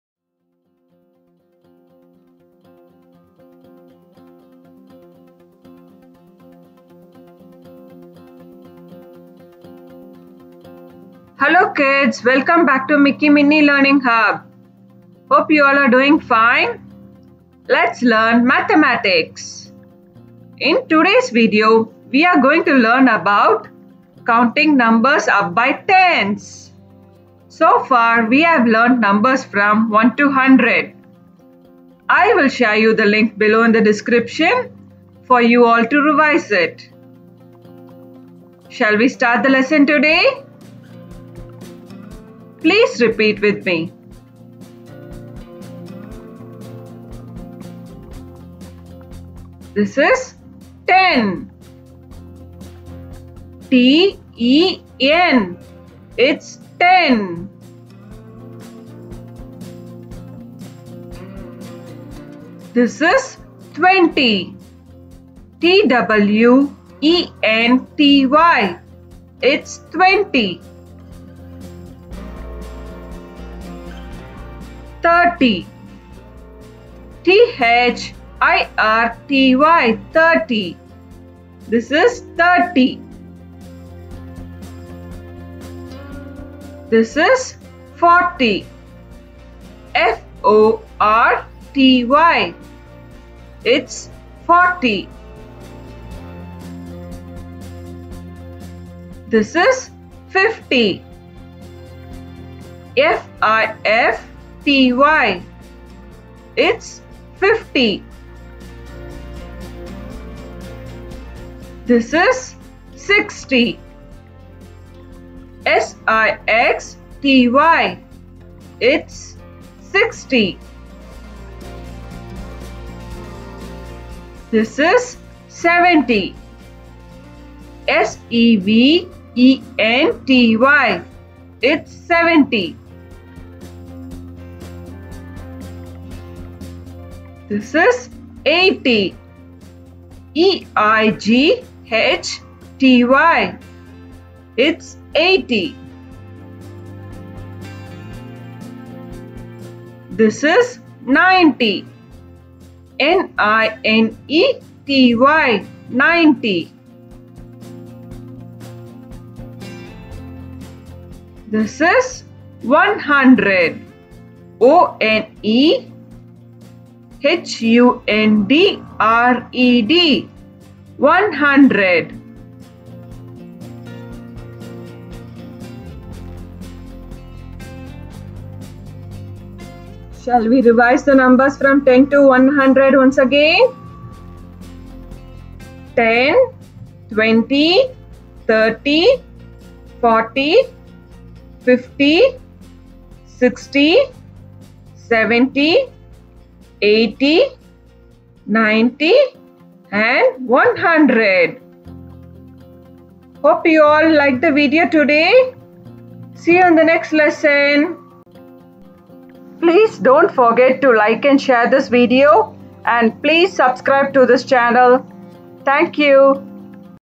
Counting Numbers Up By Tens in English | Lets Learn Numbers from 10 to 100 for Children
In this Video, we are going to learn Counting Numbers Up by Tens 10 - 100 for Children.